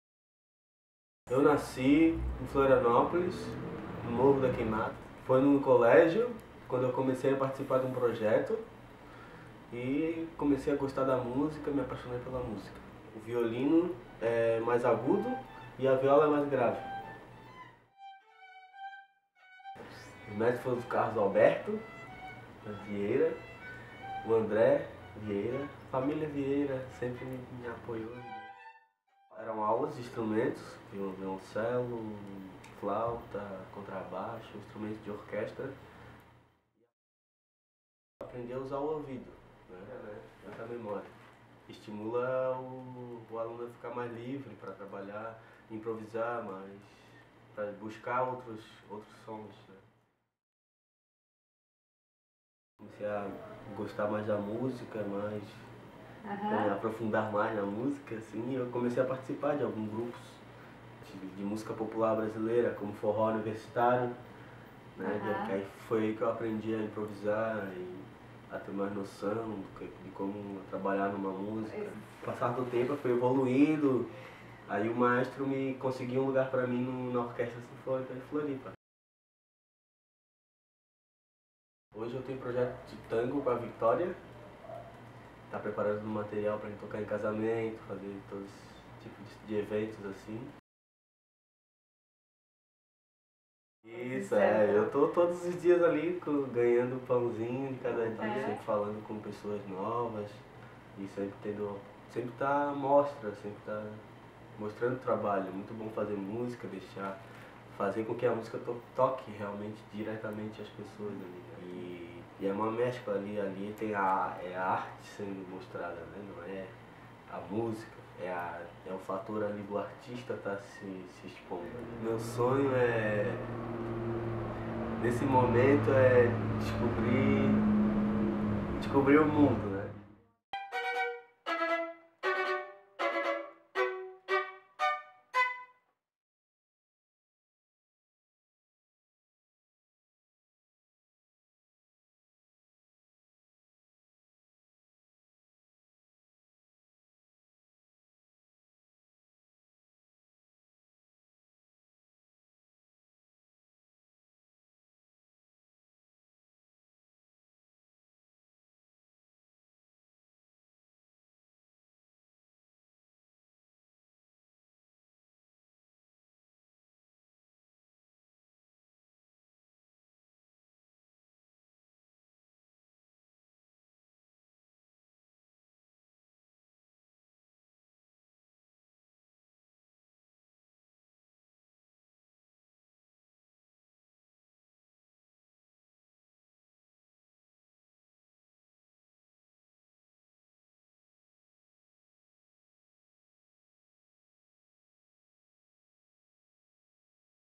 Charles Espindola, entrevista
Entrevista com o jovem e talentoso violinista das ruas e da Orquesta Sinfonica de Florianopolis.